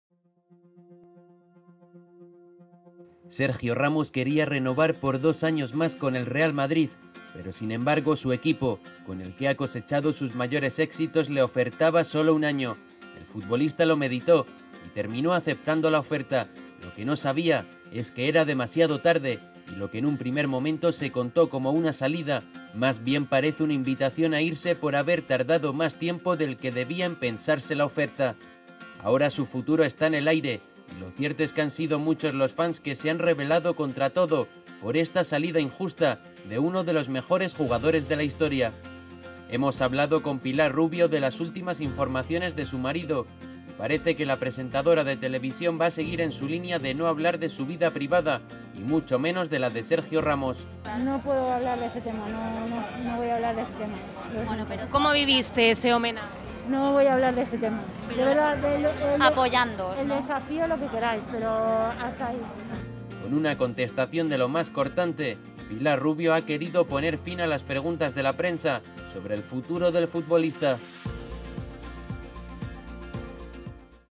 Pilar Rubio se niega a hablar del futuro de Sergio Ramos
Sergio Ramos quería renovar por dos años más en el Real Madrid, pero sin embargo, su equipo, con el que ha cosechado sus mayores éxitos parece que no le ha esperado. Ahora su futuro está en el aire y lo cierto es que han sido muchos los fans que se han revelado contra todo por esta 'salida' injusta de uno de los mejores jugadores de la historia. Hemos hablado con Pilar Rubio de las últimas informaciones de su marido y parece que la presentadora de televisión va a seguir en su línea de no hablar de su vida privada y muchos menos de la de Sergio Ramos.

(Fuente: Europa Press / Real Madrid)